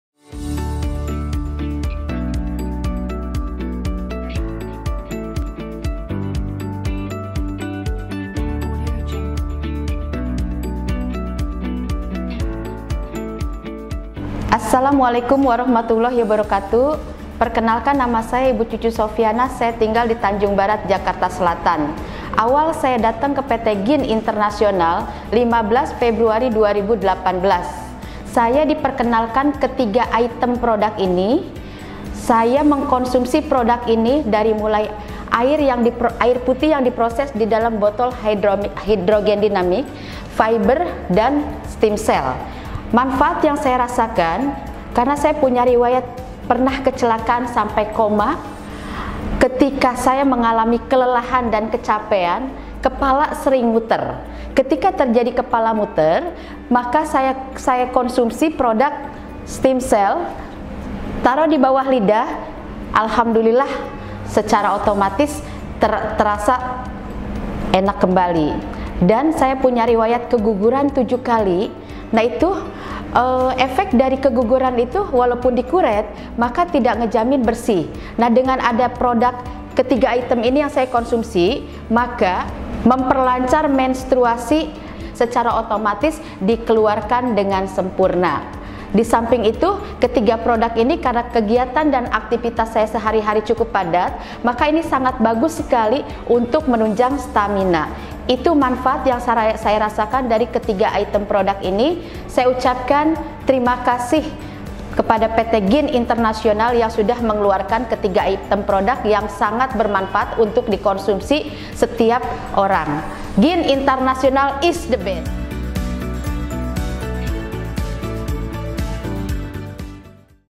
APA YANG MEMBUAT KESEHATAN IBU INI PULIH KEMBALI SETELAH SEBELUMNYA MENGALAMI KEGUGURAN 7 KALI
Ibu Cucu dari Jakarta memiliki riwayat sakit kepala yang terasa memutar ketika sedang kelelahan akibat pernah mengalami accident, bahkan Ibu Cucu merasakan siklus menstruasi beliau tidak stabil, karena pernah mengalami 7 kali keguguran, setelah diperkenalkan 3 produk dari GIN International yaitu Hydrogin Dynamic, glamore Double Stem Cells, dan glamore Nutri Fiber kini Ibu Cucu jauh lebih baik. Bagaimana kisahnya? Check this Video!